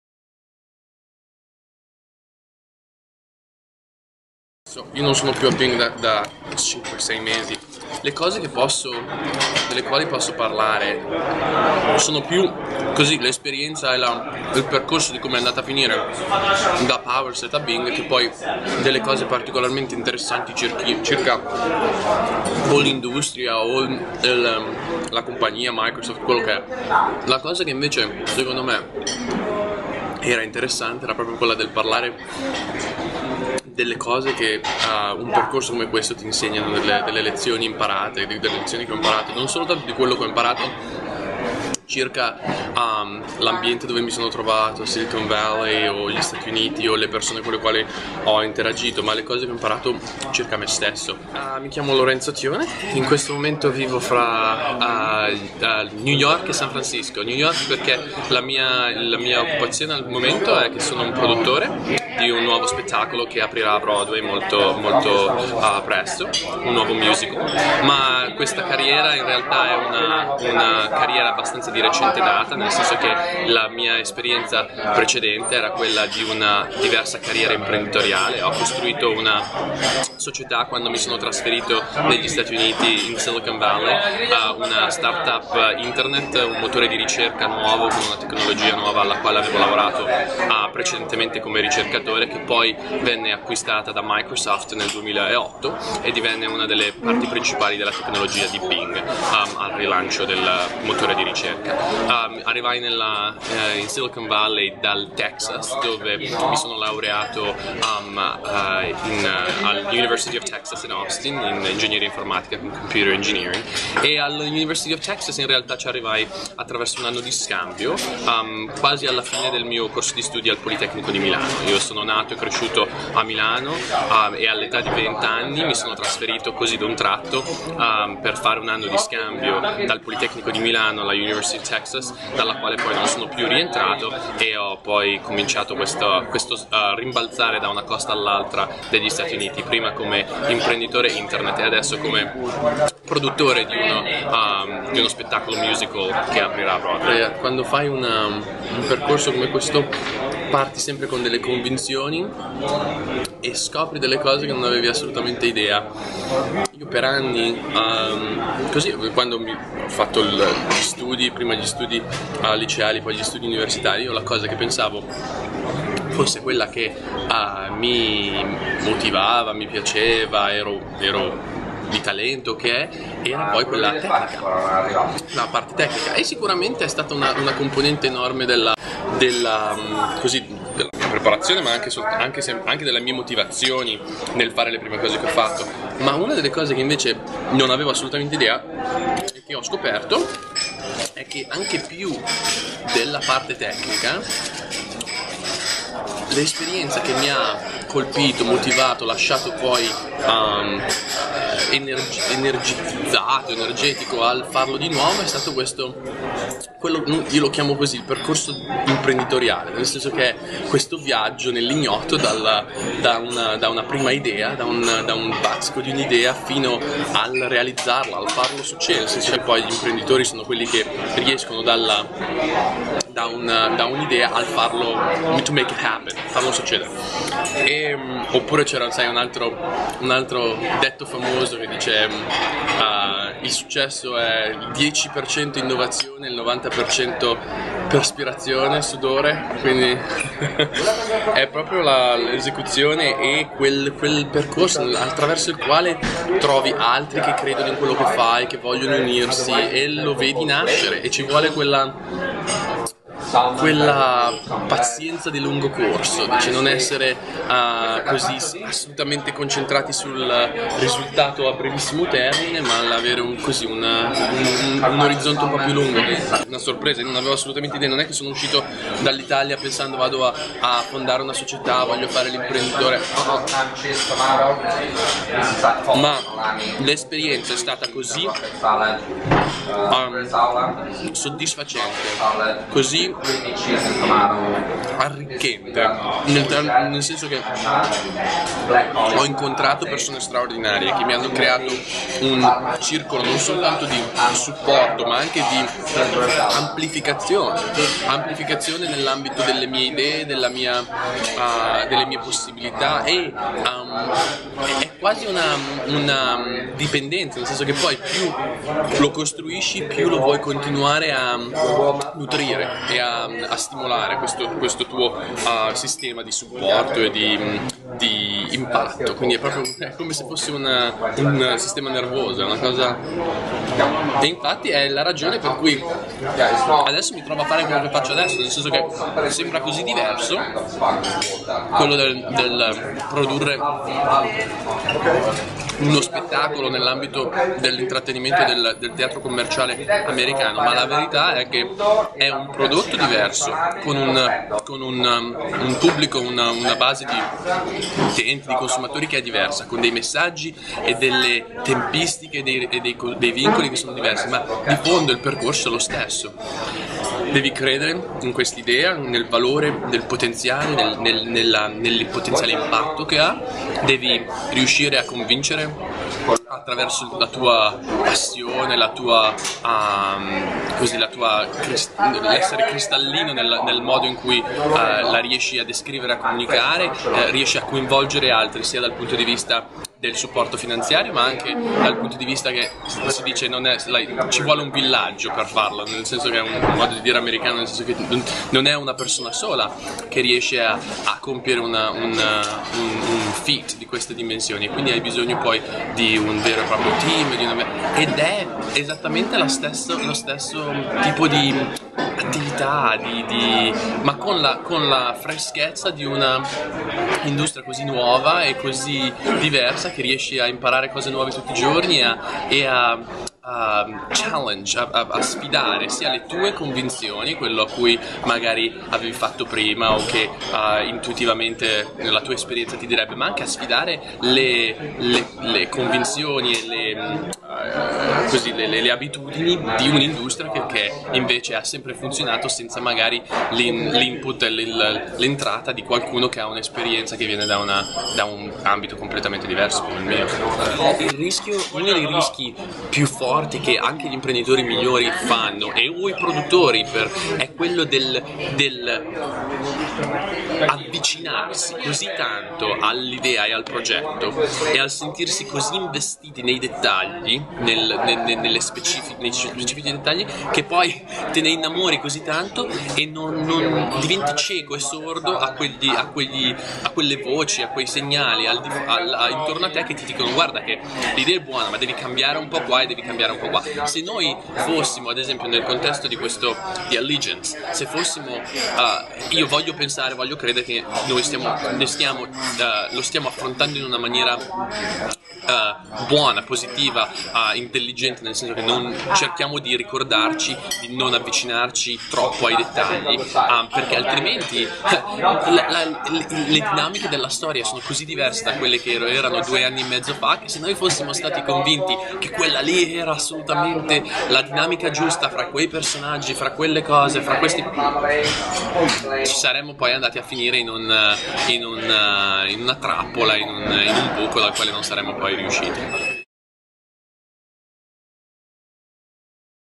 Italiani di Frontiera, Lorenzo Thione da Bing a Broadway: ecco cosa ho imparato
Lorenzo Thione, uno dei personaggi di maggior successo fra i giovani italiani di Silicon Valley, racconta a Italiani di Frontiera cosa ha impatto dalla sua straordinaria esperienza. Comasco d'origine,, arrivato negli USA dal Politecnico di Milano, dopo aver gettato "soltanto" le basi del motore di ricerca Bing venduto a Microsoft per 100 milioni di dollari, ha trasferito il proprio talento in campo artistico, con la produzione del musical "Allegiance".